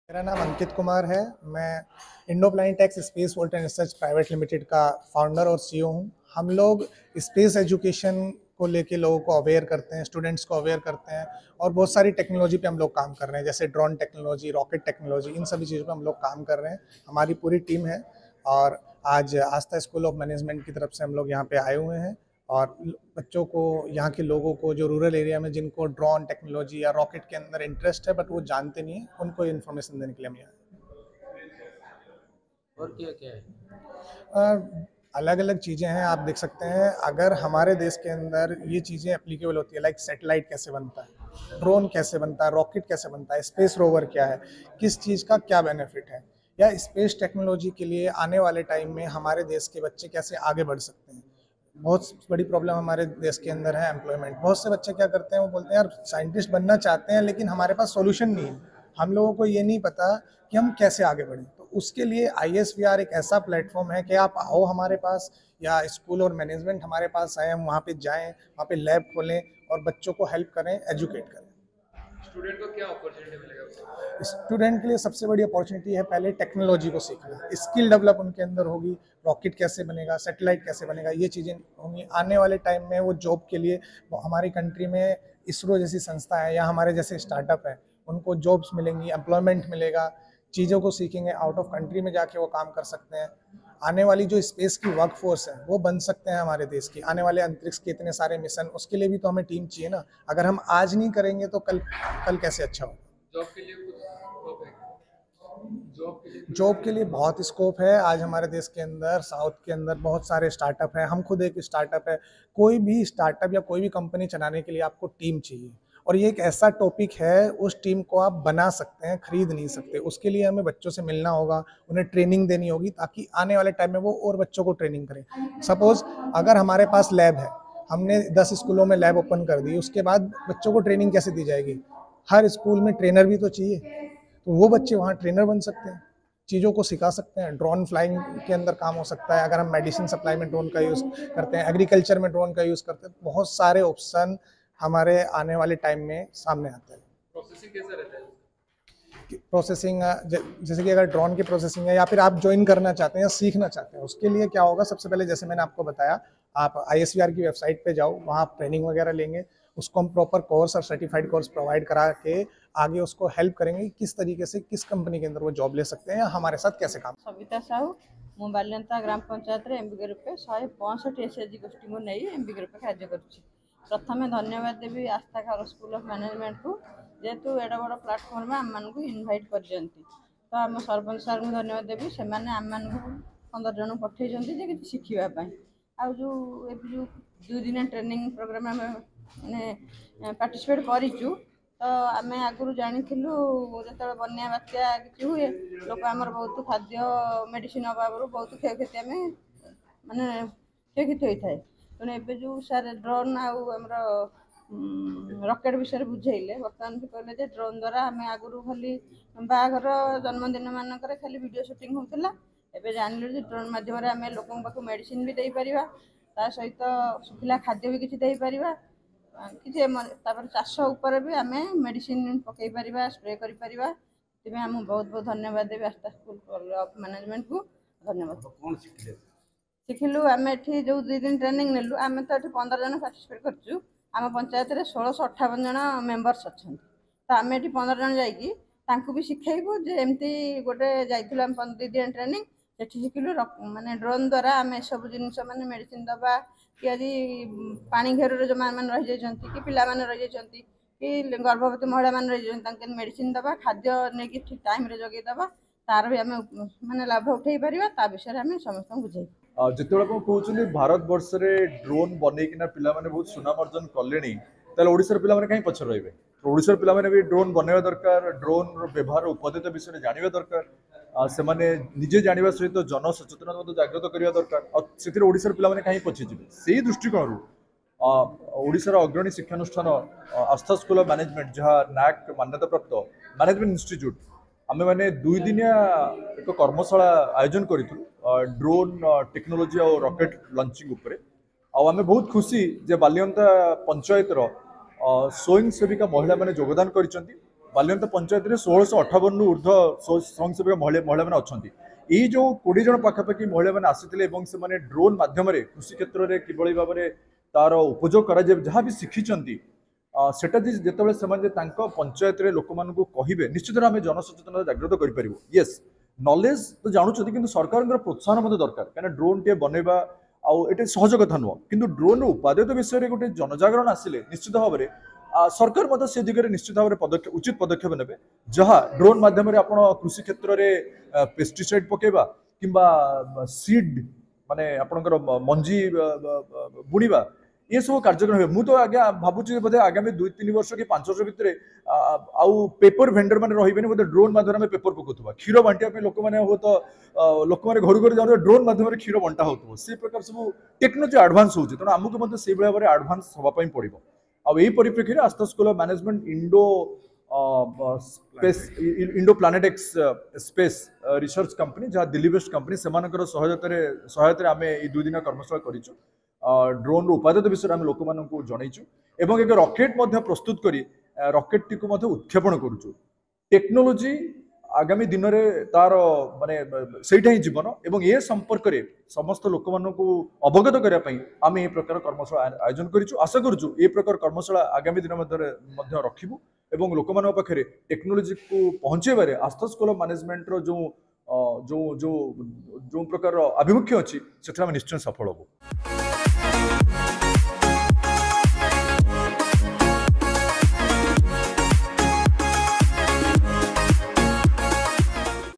ବାଲିଅନ୍ତାରେ ମହିଳାଙ୍କୁ ଡ୍ରୋନ୍ ଉଡାଣର ବ୍ୟବହାର ଓ ଉପକାରିତା ସମ୍ପର୍କରେ ସଚେତନତା କାର୍ଯ୍ୟକ୍ରମ ଅନୁଷ୍ଠିତKhordha
ବାଲିଅନ୍ତାରେ ମହିଳାଙ୍କୁ ଡ୍ରୋନ୍ ଉଡାଣର ବ୍ୟବହାର ଓ ଉପକାରିତା ସମ୍ପର୍କରେ ସଚେତନତା କାର୍ଯ୍ୟକ୍ରମ ଅନୁଷ୍ଠିତ l Khordha